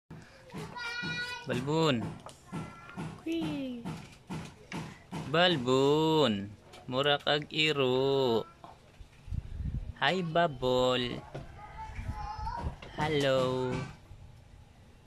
Dog guard A House Alarm and CCTV
Loving and awesome dog. Our partner in the house against illegal entry.